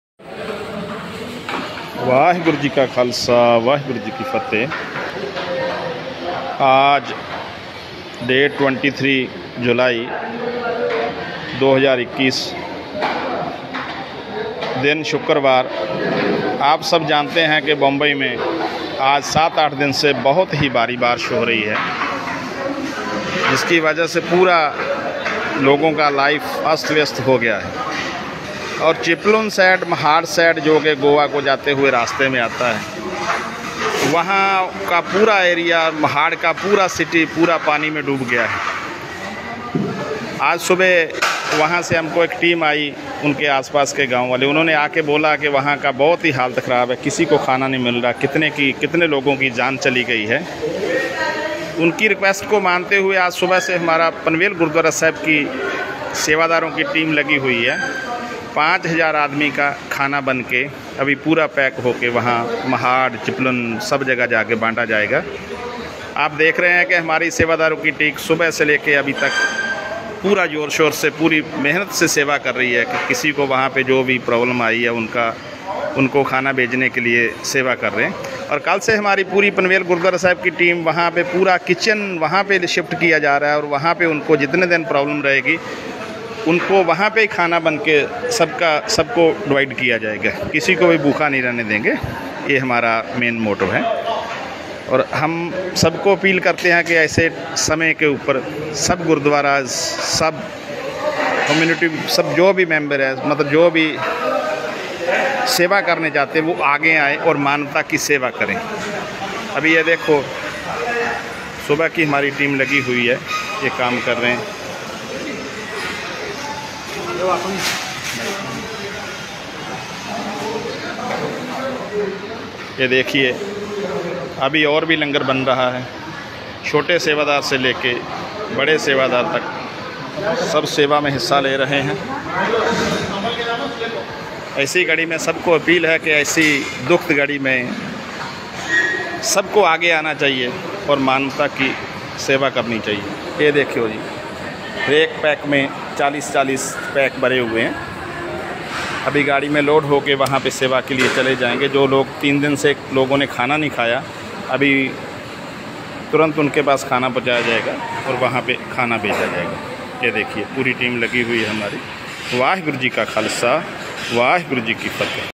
Sikh community helping people who are stuck in flood at Chiplun, Mahad | Bombay Radio Broadcast
Sikh community helping hands Always ready to help any one any where this (langer) food will bee served to the people those who are stuck in flood at Chiplun Mahad and their houses are in water.